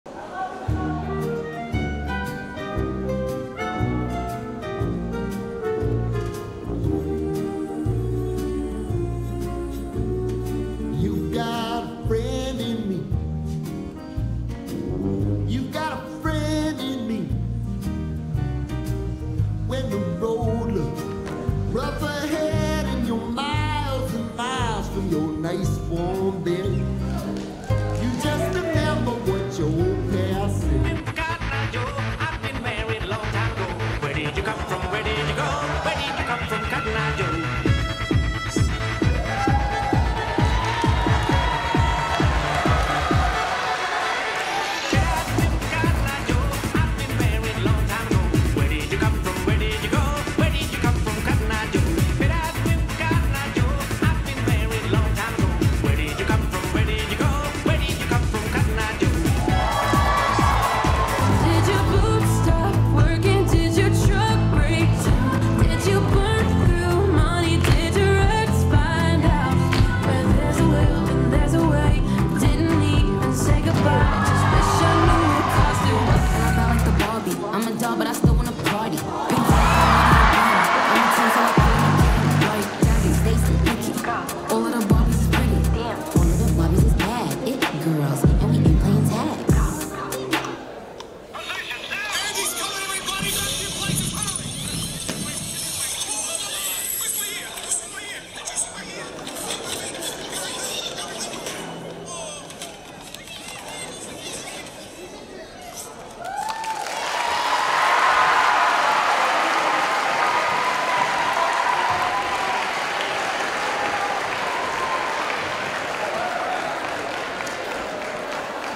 Talent Show - Freshmen Dance
The freshmen were given a few hours to prepare this dance for the homecoming competition.  There theme was Toy Story